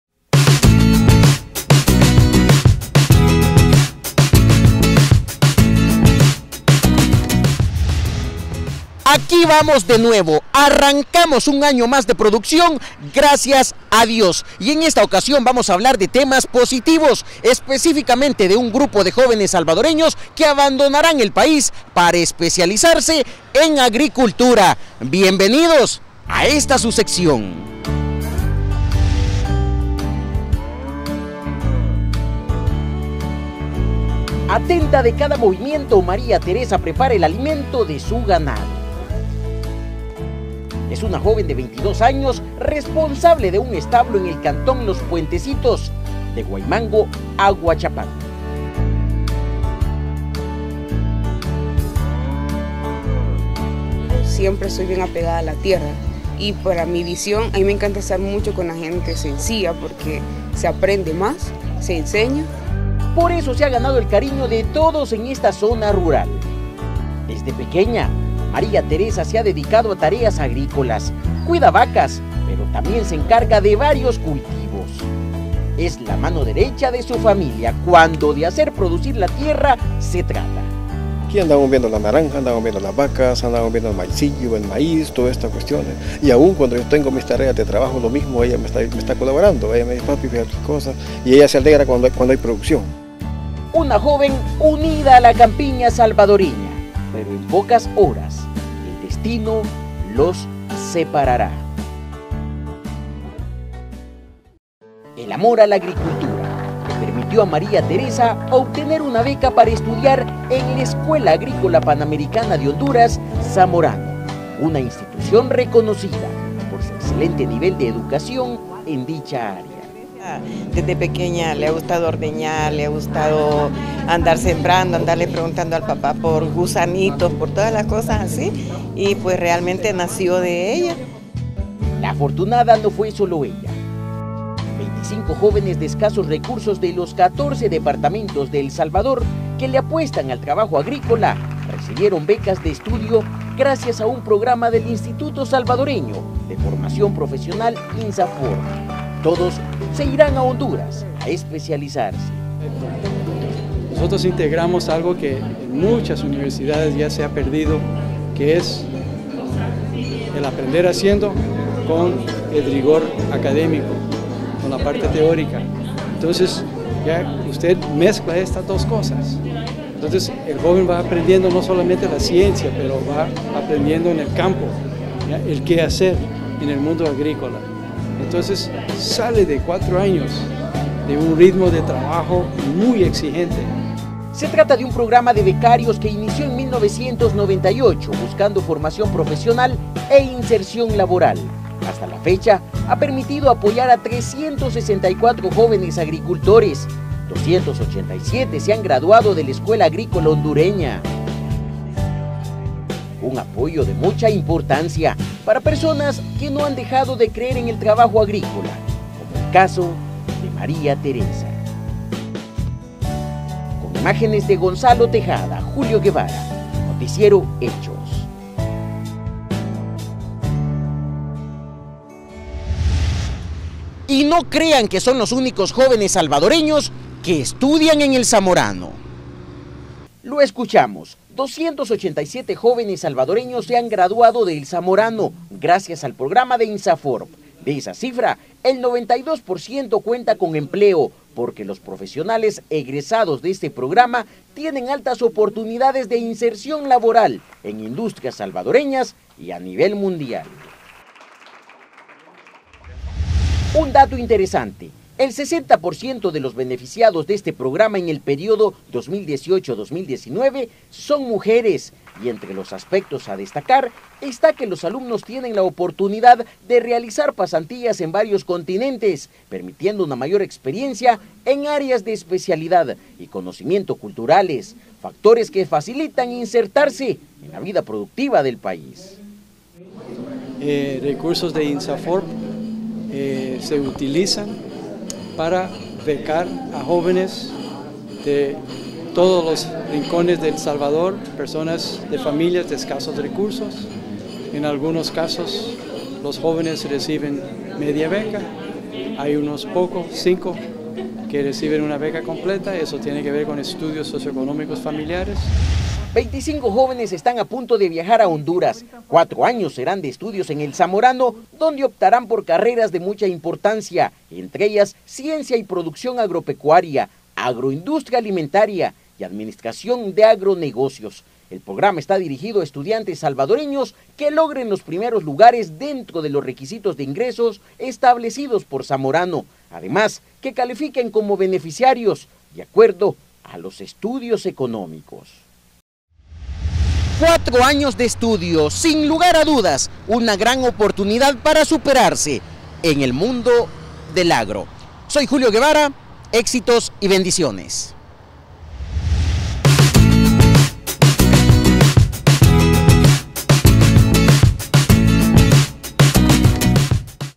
EL AGRO - BECAS PARA JÓVENES AGRICULTORES
En la sección El Agro conoceremos la historia de un grupo de jóvenes que podrán continuar con su carrera agrícola gracias a una beca de estudios.